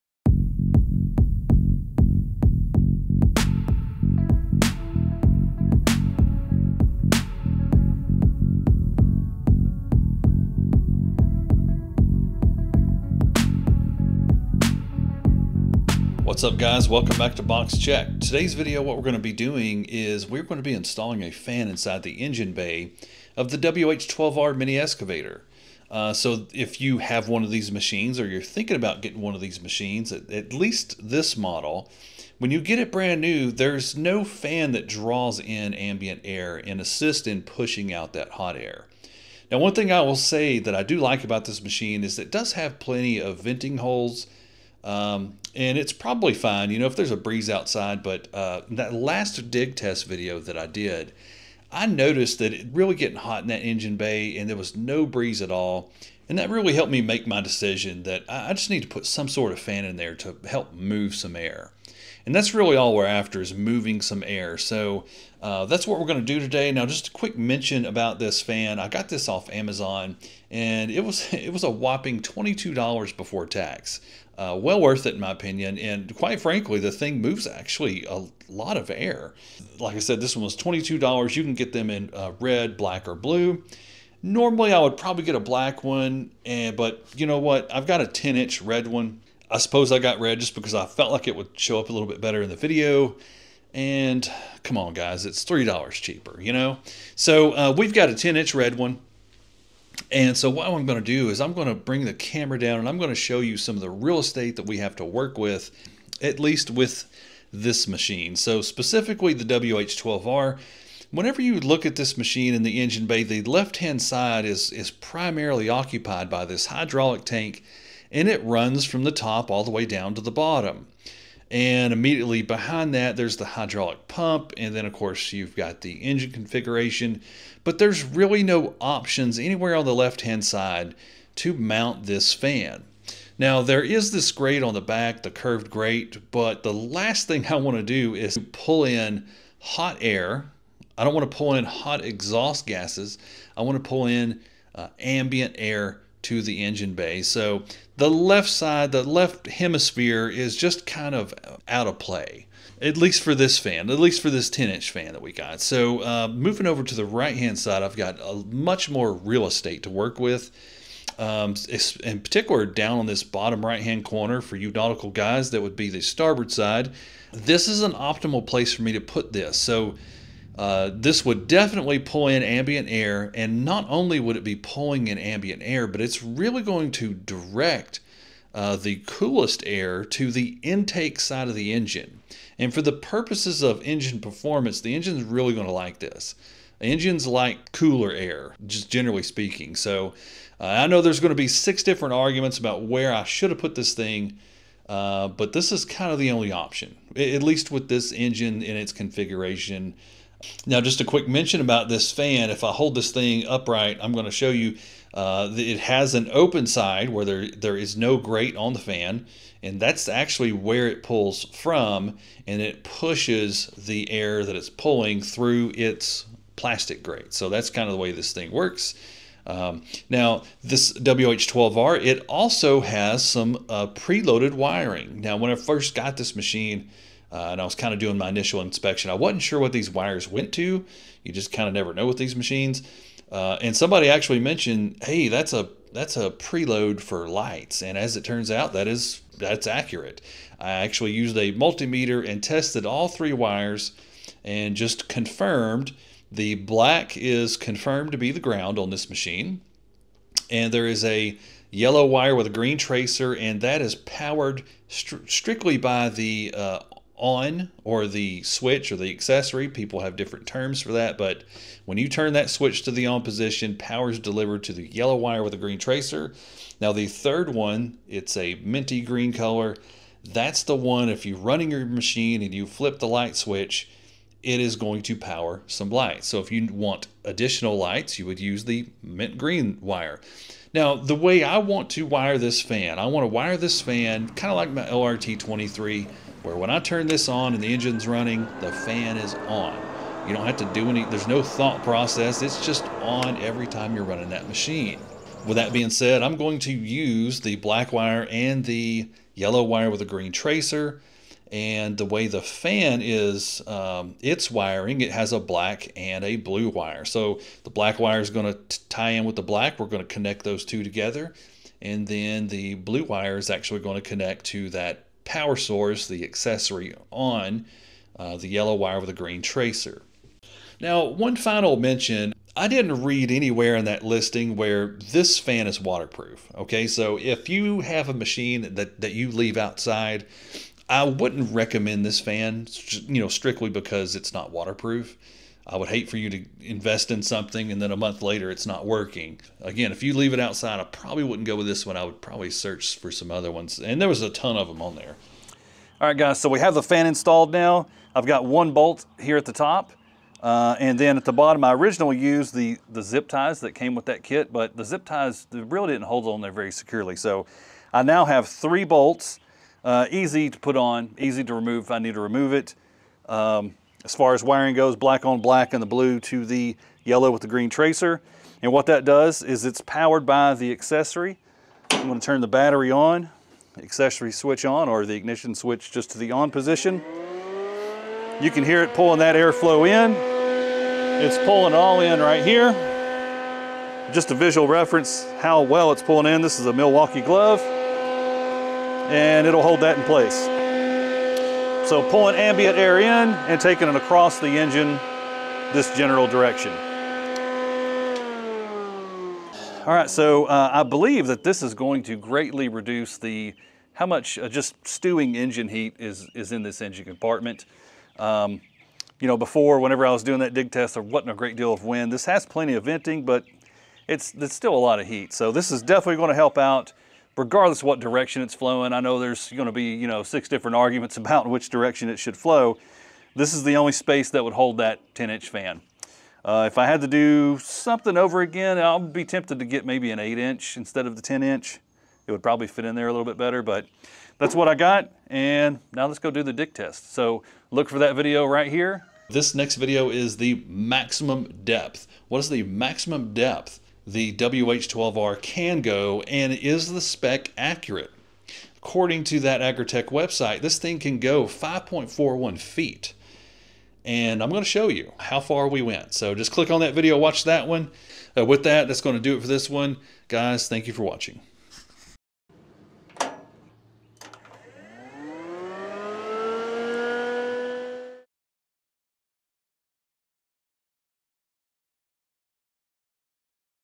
Engine Bay Cooling Fan Install - AGT WH12R Chinese Mini Excavator
In this video, we walk through the process of installing a new engine bay cooling fan on the AGT WH12R Chinese mini excavator. If you own a cheap Chinese mini excavator or are considering purchasing one, this step-by-step guide is perfect for you! We’ll show you how to improve your excavator’s cooling system, prevent overheating, and keep your machine running smoothly under heavy use.

The AGT WH12R mini excavator is an affordable, compact machine, but like many budget mini excavators, it can benefit from a few upgrades. Installing a new cooling fan can boost engine performance, extend the lifespan of your Chinese mini excavator, and help maintain efficiency during long work hours.

Whether you’re dealing with overheating issues or just want to improve the reliability of your Chinese mini skid steer or mini excavator, this video has you covered. We cover the tools and parts you’ll need, the installation process, and key tips for maintenance to ensure your excavator is always running at peak performance.

🔧 Chinese mini excavator maintenance
🛠️ Engine bay cooling fan install
💸 Affordable mini excavator upgrades

AGT WH12R Chinese Mini Excavator
H12, QH12, AGT LRT23